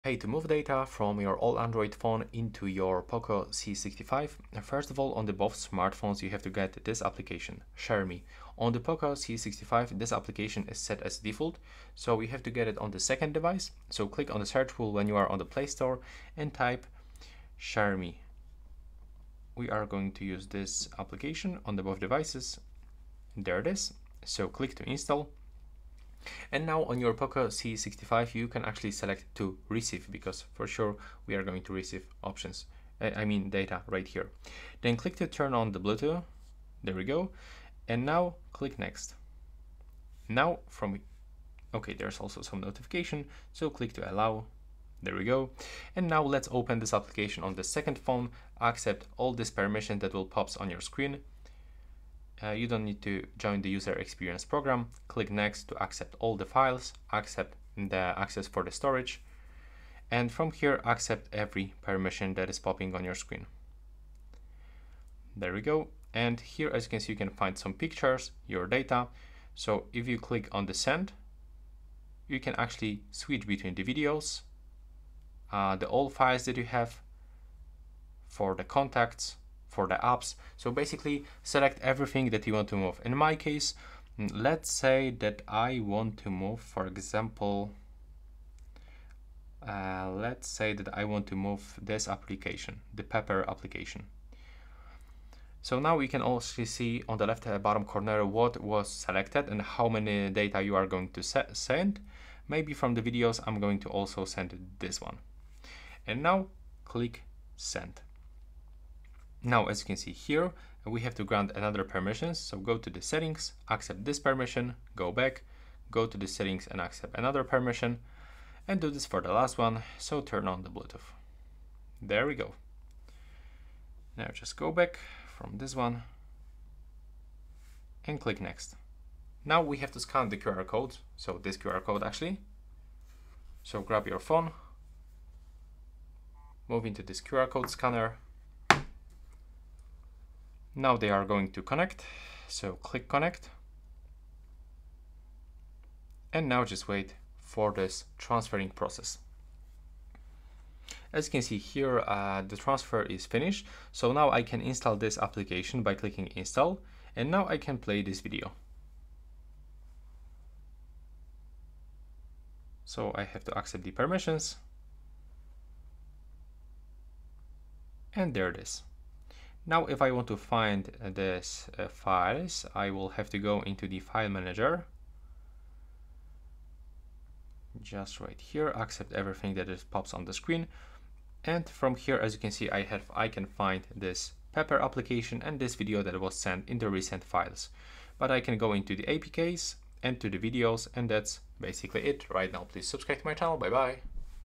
POCO C65: Transfer Data from Old Android Phone - Copy Photos, Videos, Music
Effortlessly transfer data from your old Android phone to your POCO C65 using ShareMe with our step-by-step guide in this video! Learn how to copy photos, videos, and music seamlessly, ensuring a smooth transition and preserving your important files on your new POCO device. Make the most of ShareMe for a hassle-free data transfer experience!